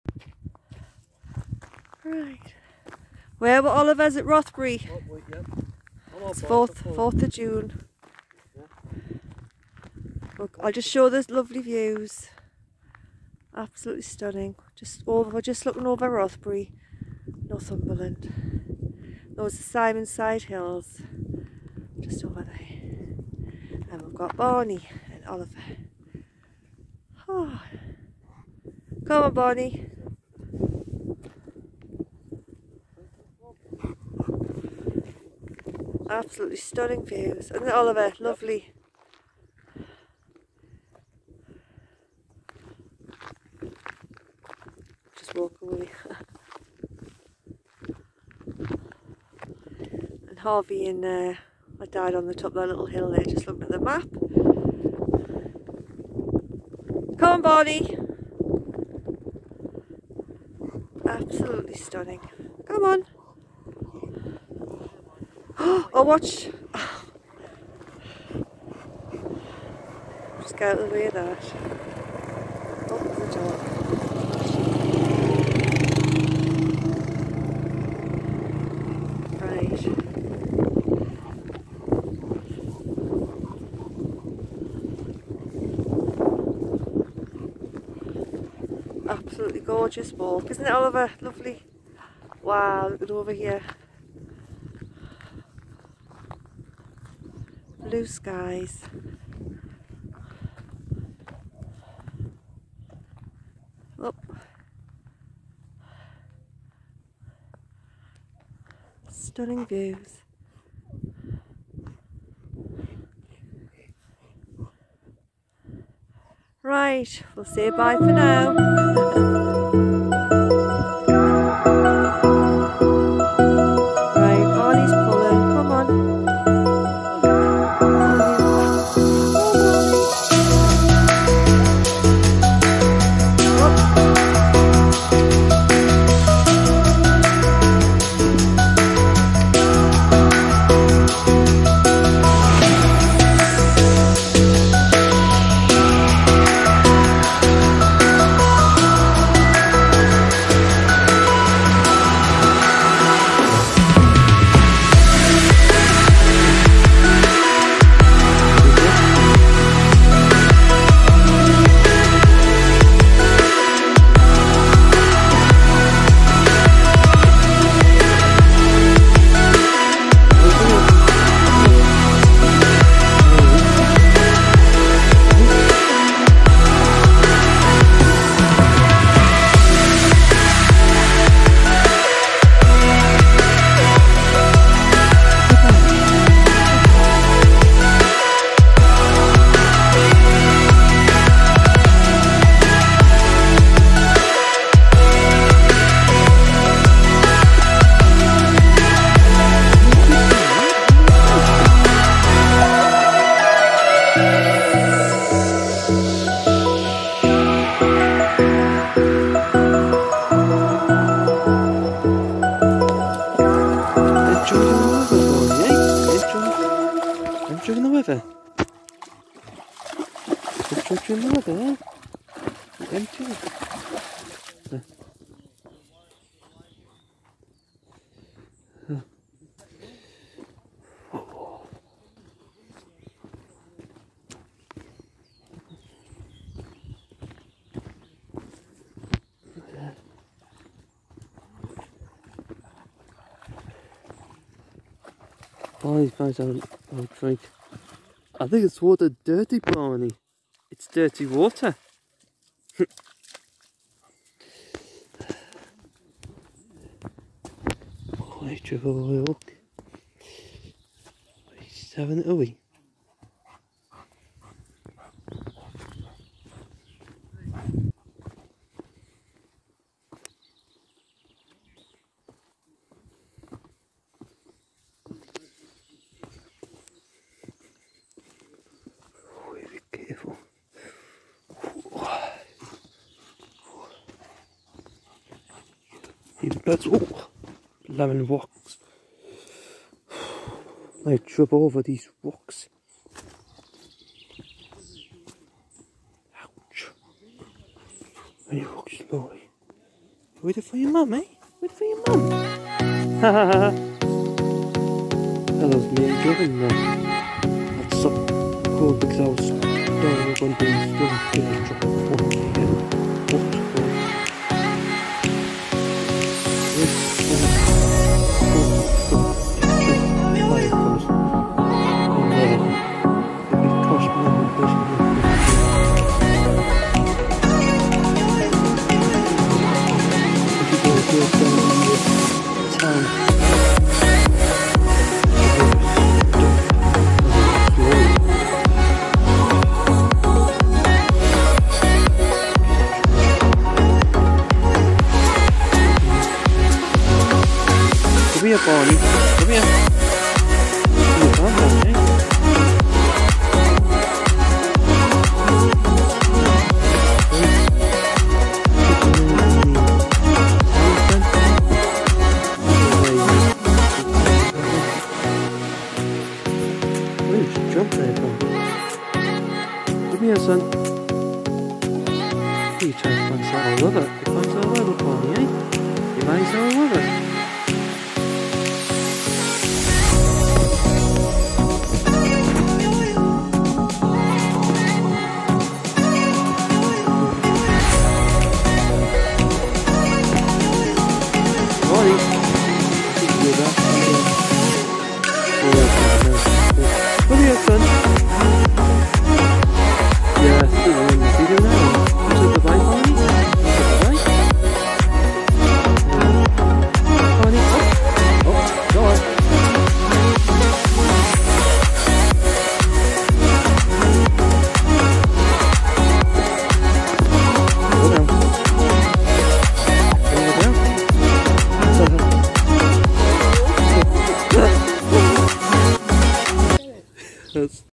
Barney Adventures #36, Barney walking on Bleaberry Hill, Rothbury, Northumberland.
Barney enjoyed his day out at Bleaberry Hill in Northumberland, Barney Adventures #37 will coming soon.